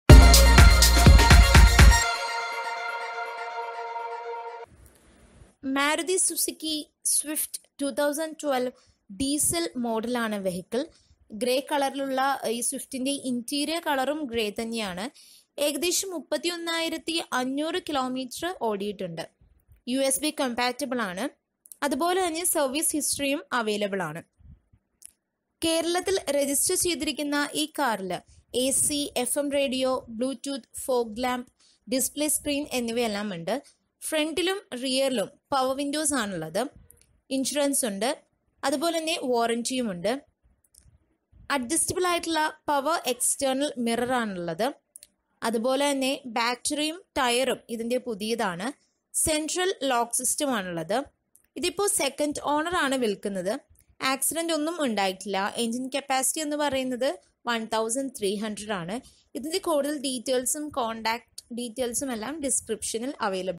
Used Car Kerala || Maruti Suzuki Swift || 31,000 km
Brand   Maruti Suzuki
Model   Swift
Year    2012
Fuel    Diesel
KM driven 31,500 km

Registration Transfer: Yes

Insurance: Yes

Color: Grey

Air Conditioning: With Heater

USB Compatibility: Yes

Aux Compatibility: Yes

Service History: Available

No. of Owners: Second

Registration Place: KL

AM/FM Radio: Yes

Accidental: No

Automatic Boot: Yes

Flood Affected: No

Transmission: Manual

Power Windows: Front & rear

Warranty: Yes

Variant: Swift VDi

Vehicle Certified: Yes

Engine Capacity/Displacement (in Cc): 1300

Fog Lamps: Yes

Display Screen: Yes

Interior color: Grey

Finance: Yes

Adjustable External Mirror: Power

Exchange: Yes

Type of Car: Hatchback

Battery Condition: New

Insurance Type: Comprehensive

Condition: Used

Power steering: Yes

Make Month: July

Bluetooth: Yes

Lock System: Central

Tyre Condition: New

Number of Airbags: None